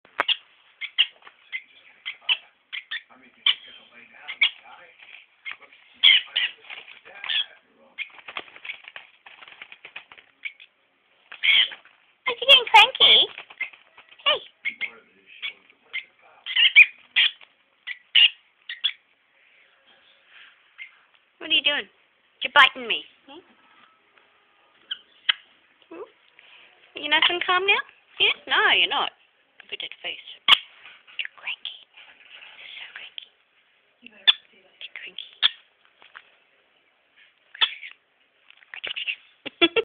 Mist tantrum
This is mist having a tanty one day.